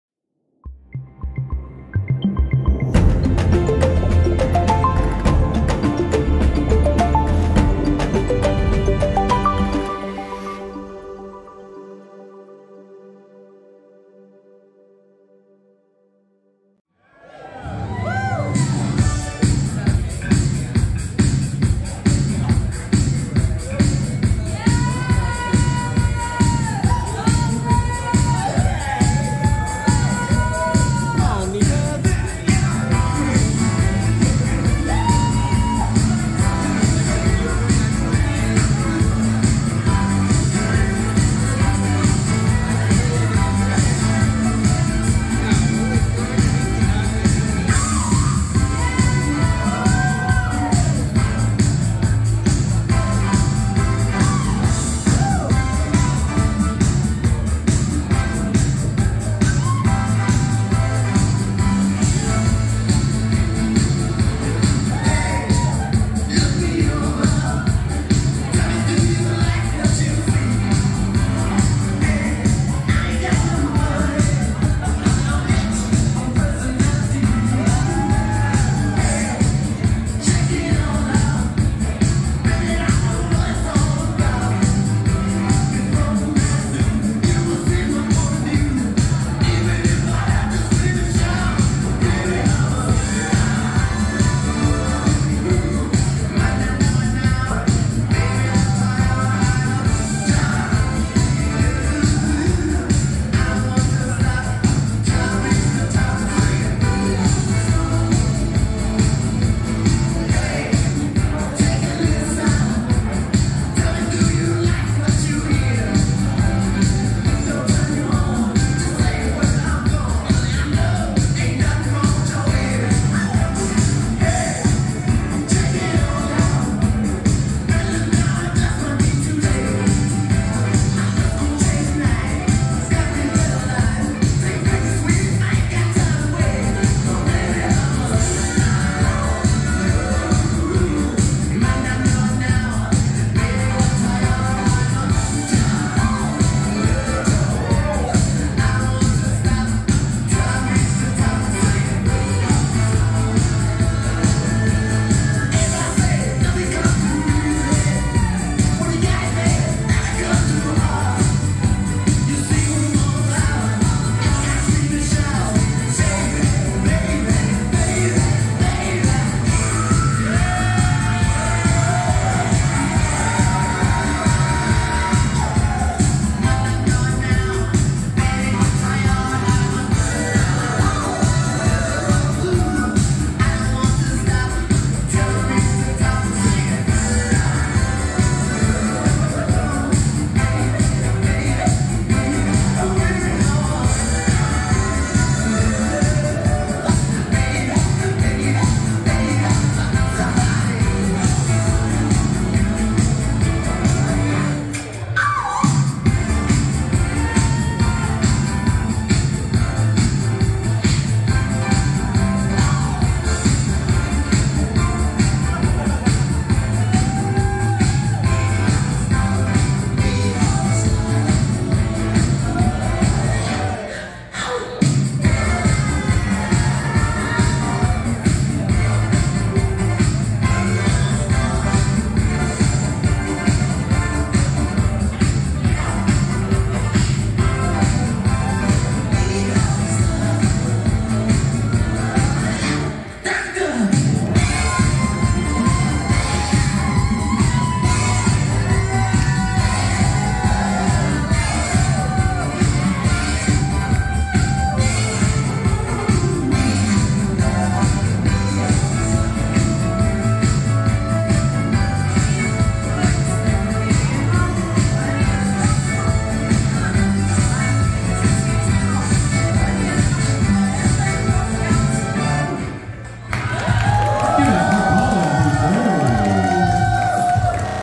Knockout Superstars (Round 1): Drag Roulette - Colin Butté
Colin Butté - Contestants are assigned a song to perform at random.

I claim no rights to music in this video.